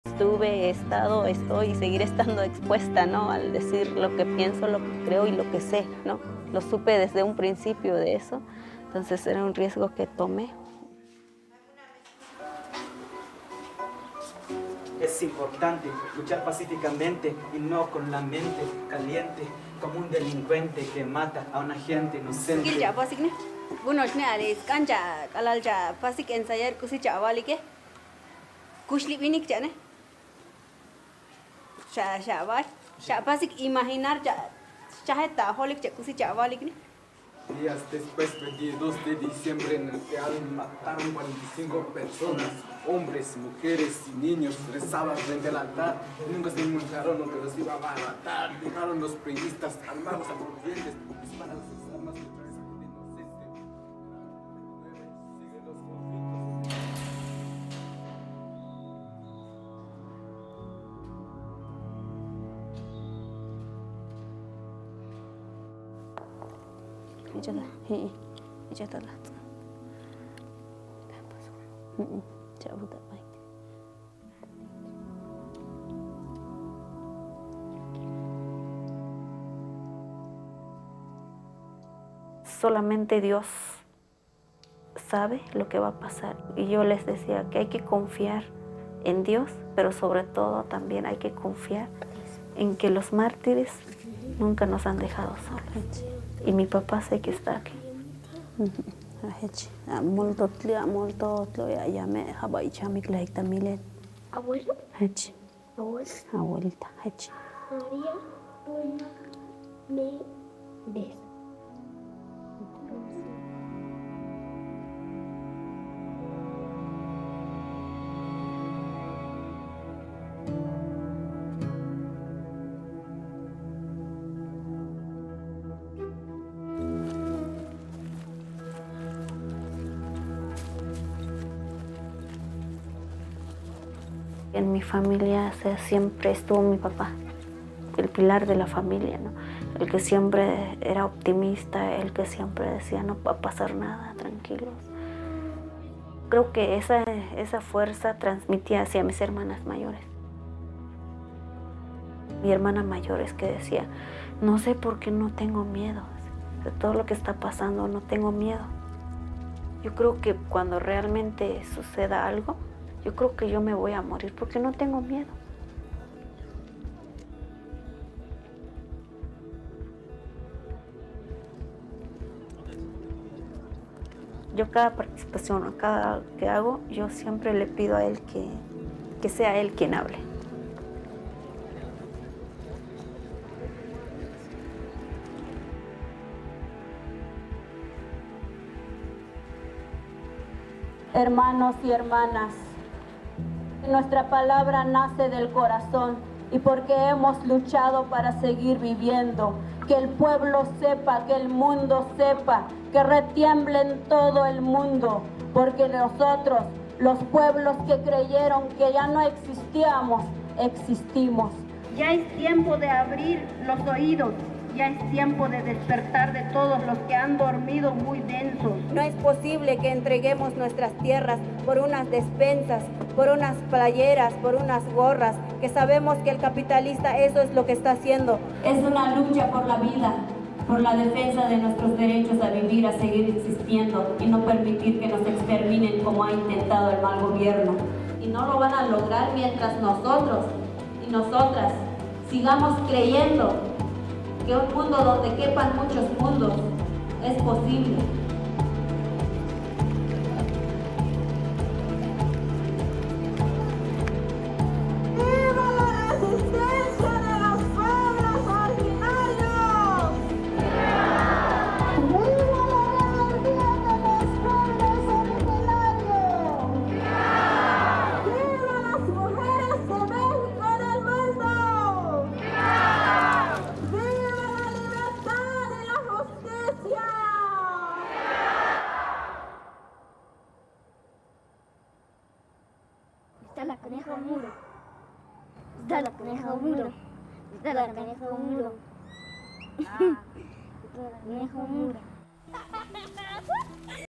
A Call to Action: Movement of Resistance of Ordinary Mexican People - Lupita
Join us in this compelling video titled "A Call to Action: Movement of Resistance of Ordinary Mexican People," where we delve into the powerful voices of everyday Mexicans standing up against injustice. Featuring heartfelt testimonies and reflections, this video captures the essence of a community's struggle for dignity and rights.

In this video, we hear from a courageous individual who shares their experiences of staying true to their beliefs despite the risks. They emphasize the importance of peaceful resistance and faith, drawing strength from their family's resilience, particularly a father who was the pillar of optimism and courage.

This movement underscores the urgency of defending land and rights against capitalist exploitation. It's a call for unity, urging communities to awaken and stand firm against oppression. The speaker passionately advocates for a world where diverse cultures and ways of life coexist.

This video is part of the WVN Film Festival, featuring award-winning human rights activists and filmmakers who use emerging technology to amplify critical social issues. Join us to witness the power of collective resistance and the unyielding spirit of ordinary people fighting for justice.

Viva the resistance of indigenous peoples! Viva the women of Mexico and the world! Viva freedom and justice!

CHAPTERS:
0:00 - Subcomandante Marcos
2:14 - María de Jesús Patricio Martínez (Marichuy)